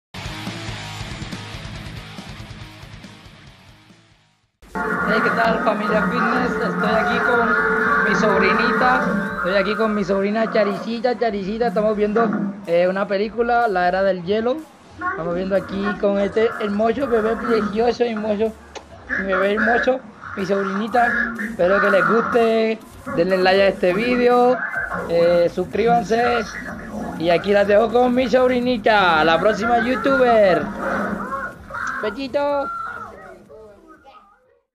Bienvenida a mi sobrinita Fitness | Jesús Arrieta
Aqui un pequeño videeo de mi sobrinita , para q la conoscan pronto sera la nueva baby fitness jj saludos.